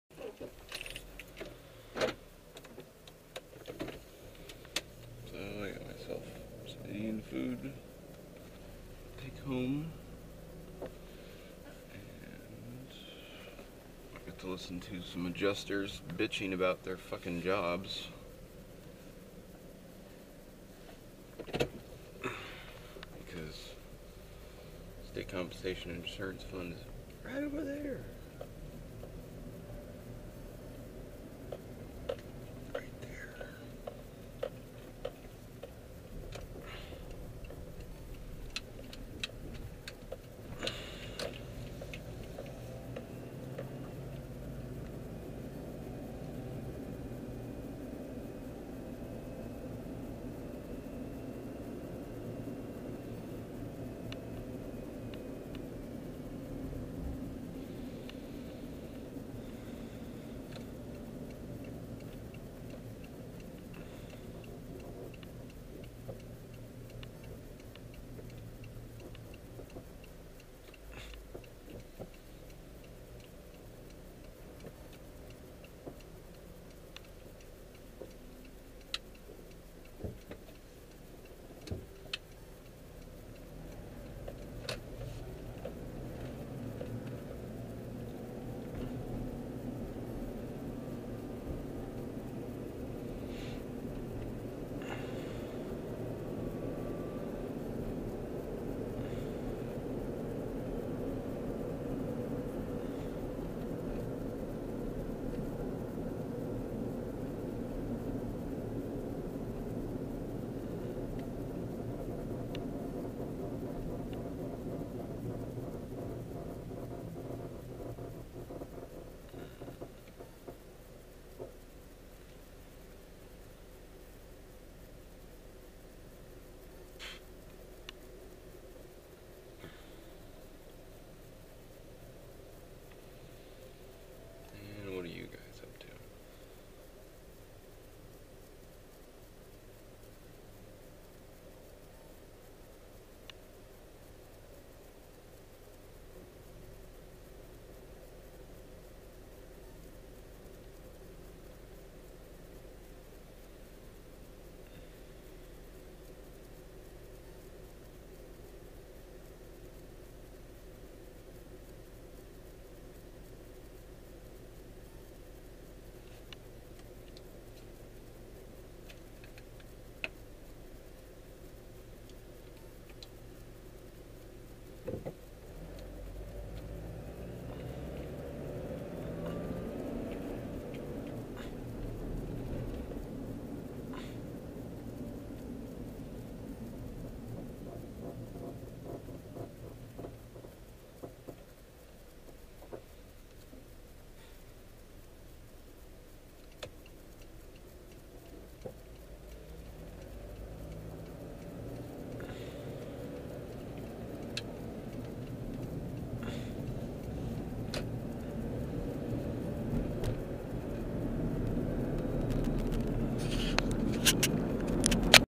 2012.10.15 Daily Driving: Medical Cannabis Errand [8/11]
Leaking face edition. Crying, sad, in pain, emotional, physical. Driving around now with MORE commentary! Sometimes even special Guests. Just an example video from my Contour HD camera to show off the video quality to friends. You might want to turn down your speakers, the microphone is lacking.